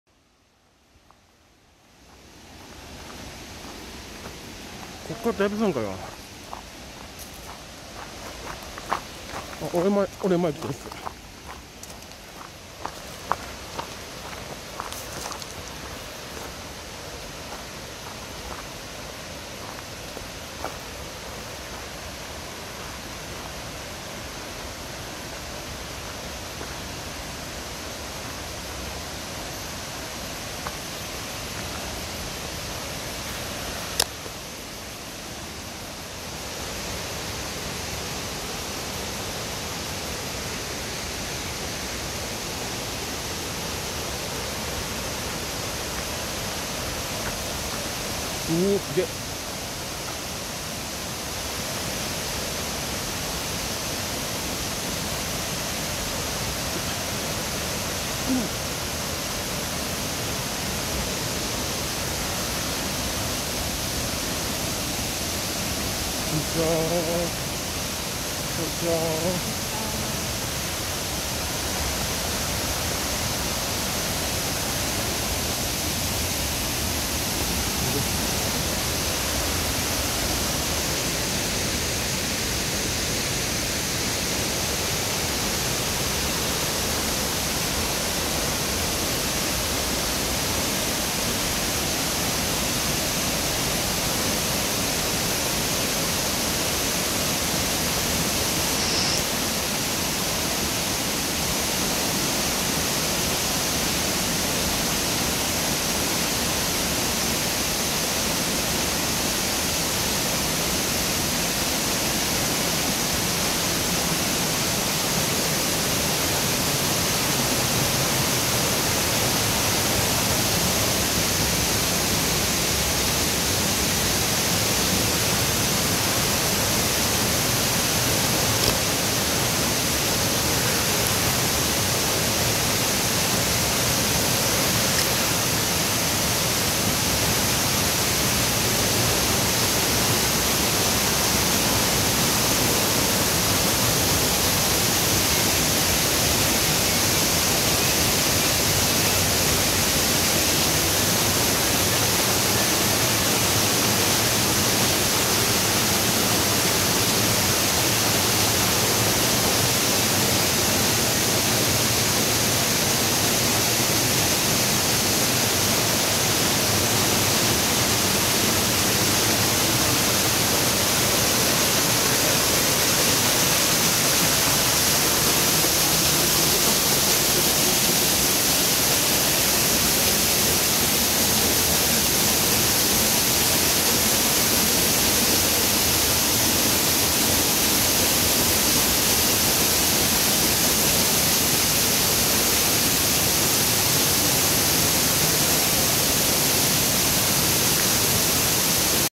2020.08.17 長野県高山村 雷滝にて (前半)
雷滝は、松川渓谷にある落差約30ｍの大滝。別名、裏見の滝とも呼ばれています
滝の裏側から流れ落ちる姿を見ることができます。雷鳴のように轟音を立てて落下する姿は息を呑む豪快さです
そこに降りる途中で　上から落ちる水滴により途中で録画が切れてしまいました！
後半では　その豪快な落下する姿を御堪能下さい